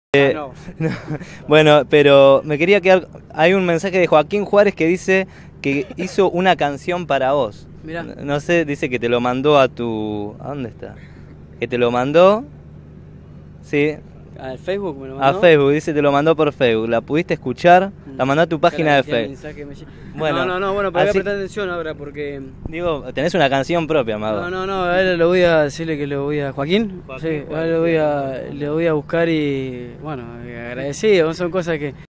Mago coria se entera de la existencia de mi canción
Este vídeo fue editado en el momento justo donde le comentan al mago de mi canción.
Todos los derechos pertenecen a sus respectivos propietarios, en especial al canal argentino Deportv.